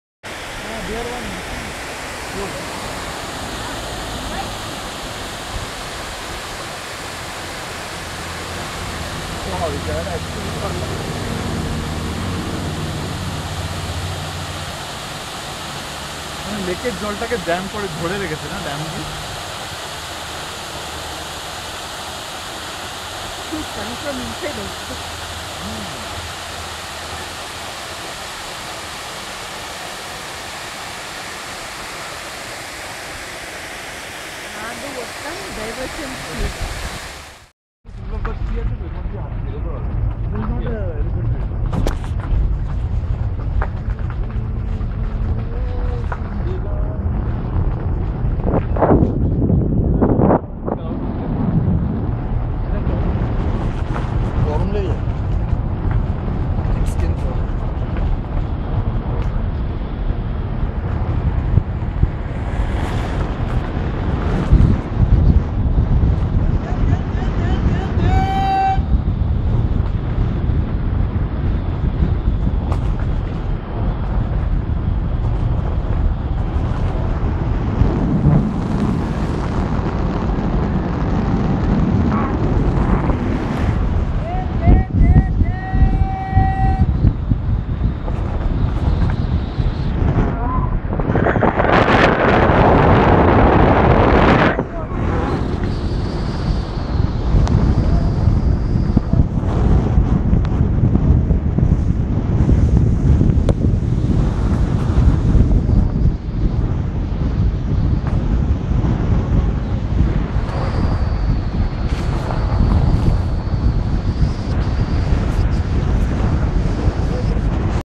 Ooty- Pykara Lake tributary and the road journey beyond!
Pykara Lake is one of the most famous lake surrounding the hill-station of Ooty. This video was shot at a place called 'Naduvattam' wherein one of the tributaries of Pykara Lake is channeled for irrigation.
The road between Naduvattam and Gudalur is surrounded by pristine forests of Deodhar  and Eucalyptus- An amazing driving experience!